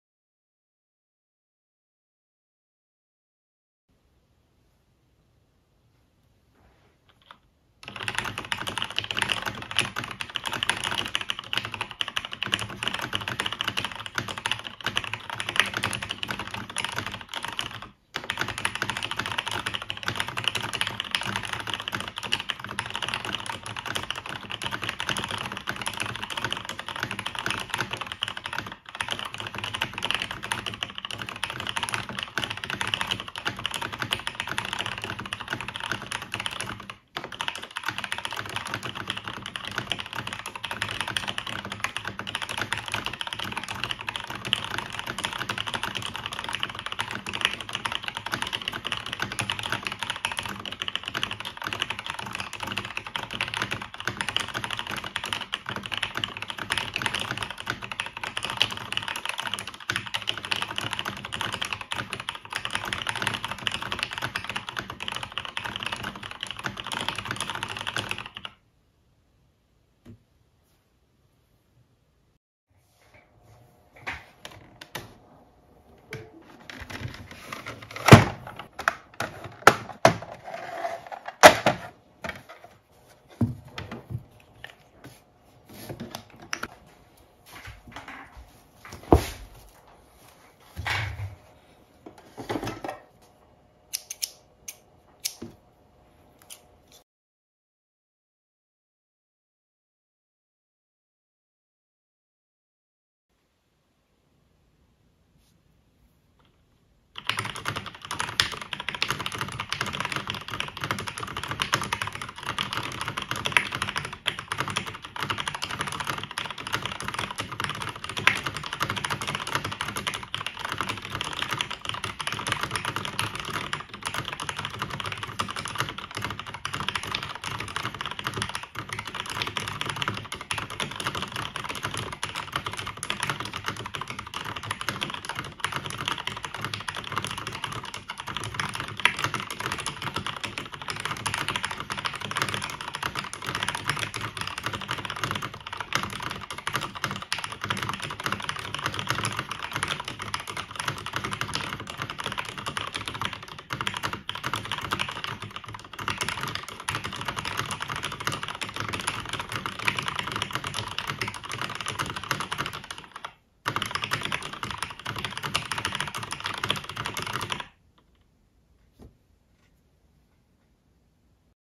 Cherry G80-1800 | MX Black | Typing Sounds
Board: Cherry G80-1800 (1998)
Mount: Plateless
PCB: Leechee 1800
Plate: None
Switch: MX Black w/ 3204 & 105 || 16mm 67g TX || Deskey's Film
Stabilizer: OG Cherry Clip-in w/ 205g0 & foam wire modded
Keycap: ePBT

Deskmat: (1st) Christmas Tea || (2nd) Yuru Nightscapes
Recorded on my iPhone (video, audio amplified 10db)

I got this board from Ascaii (thanks!). Unfortunately the blacks turned out to not be vintage, but they feel really good so I don't care. The back of the board was pushed in kind of when I first got it, so I had to use a heat gun to try to bend it back in place for me to put the aftermarket PCB in.
There's a hole on the bottom too, maybe for a badge to screw in? I'll leave it alone for now.
The layout is nice. 6u should become more popular on 65-75%'s.